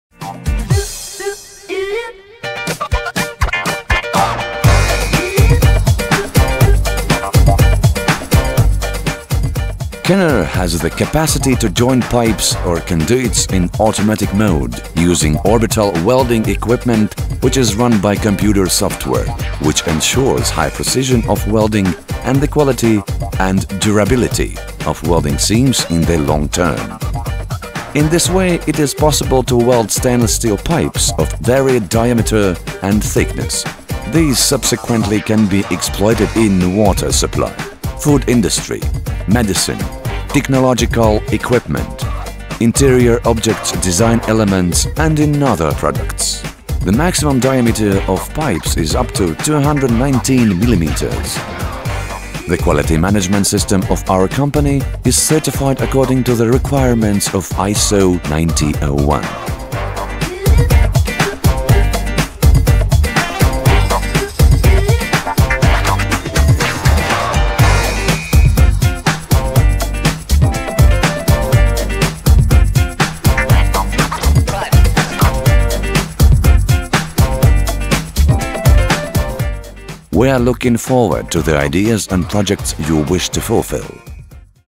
Orbital pipe welding - Kenner.lv services
KENNER offers automated joining of metal pipes using an orbital welding machine with argon, which is run by computer software; this leads to a high-precision welding, high-quality weld seams and more extended durability. Using this method it is possible to weld stainless steel pipes of different diameter and thickness, which later can be used in water supply, food processing, pharmaceuticals, technological equipment, interior objects, elements of design and other articles. The maximum diameter of pipes can be 219 mm.
Samples can be seen in our production facilities.
You are welcome to make us know your ideas and projects you wish to realize!